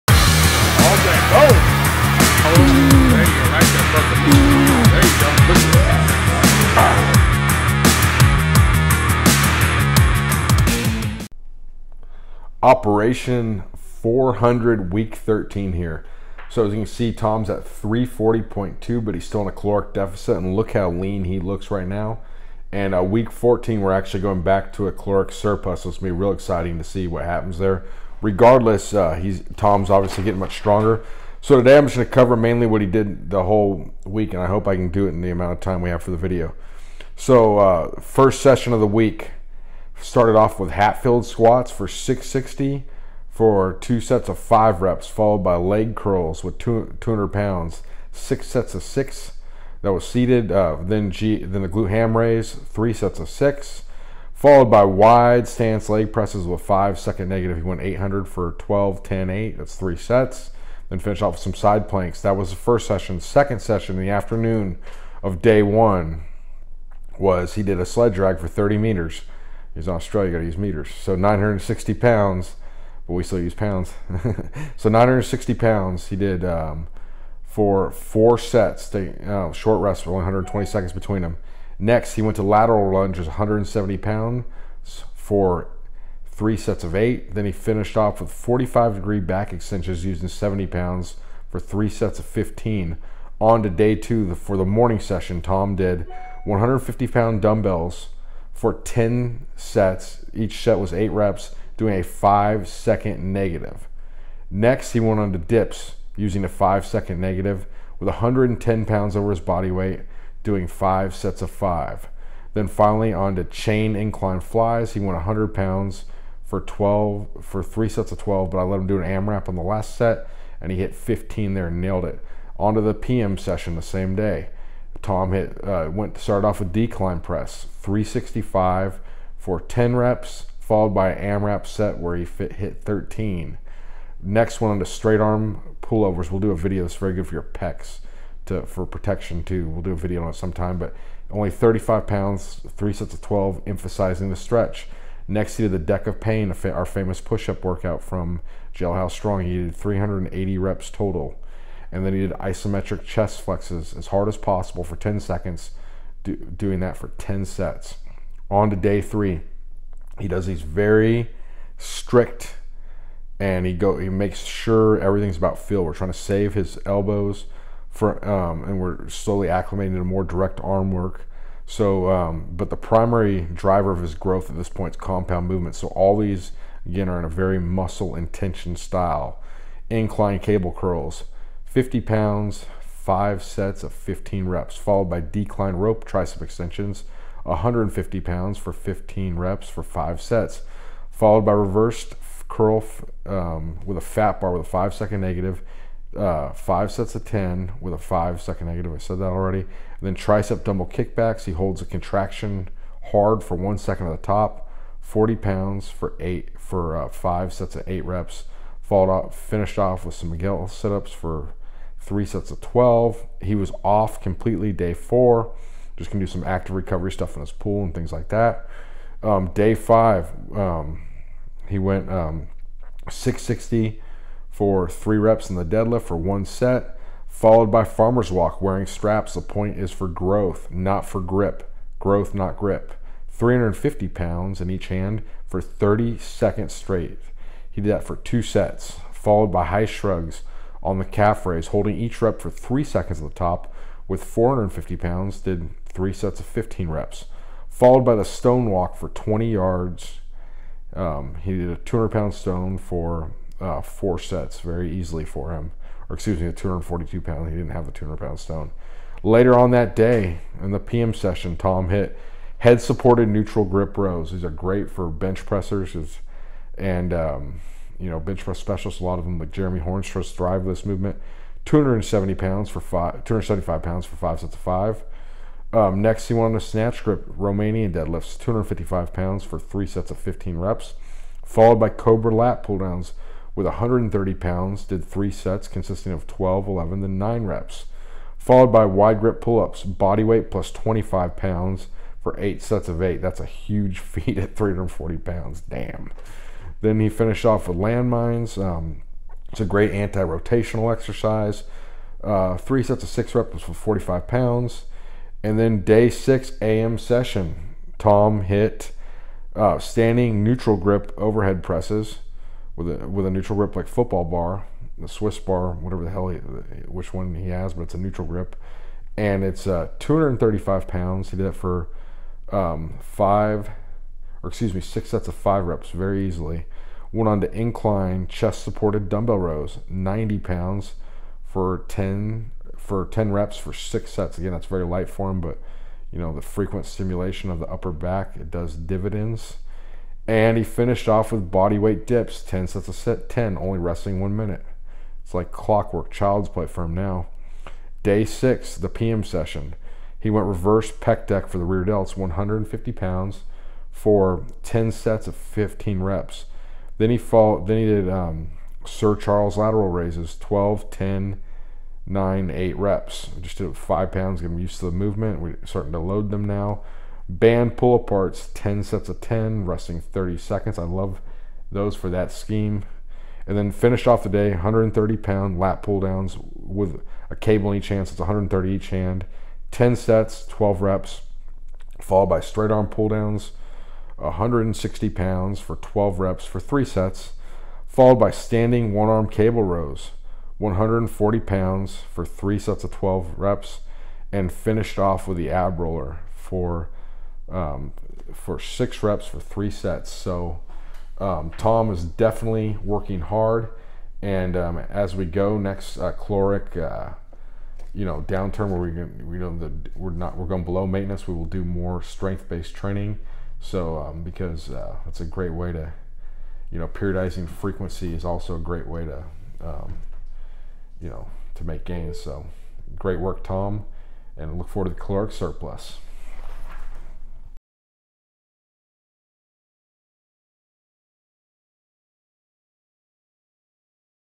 Operation400 Week 13
In this weeks video Josh breaks down Tom's training in explicit detail discussing every single set, rep & weight used.  This is Tom's last week of a negative calorie balance--next week the surplus begins!